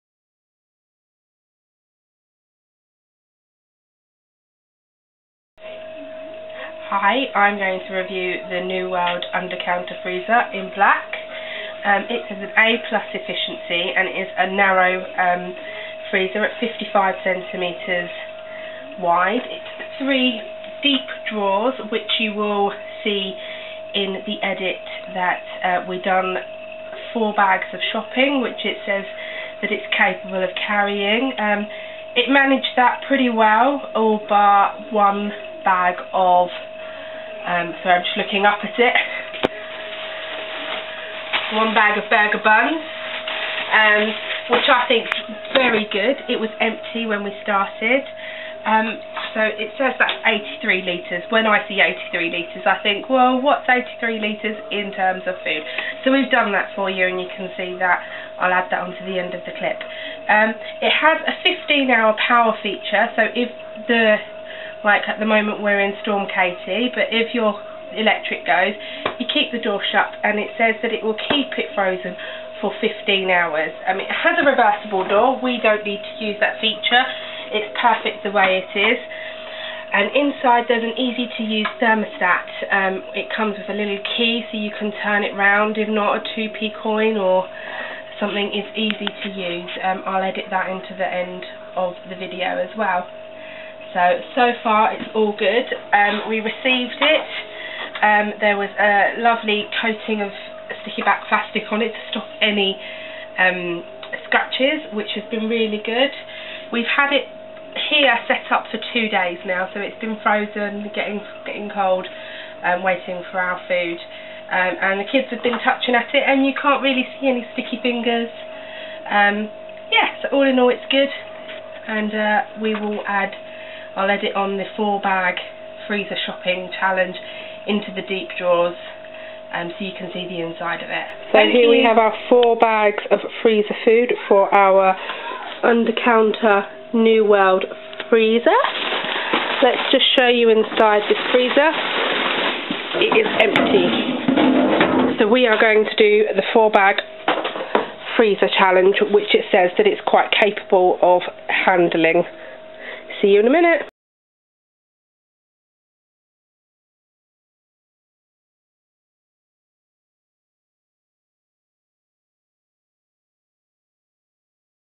New world undercounter freezer review
68 litres?? whats that look like??
4 bag challenge and review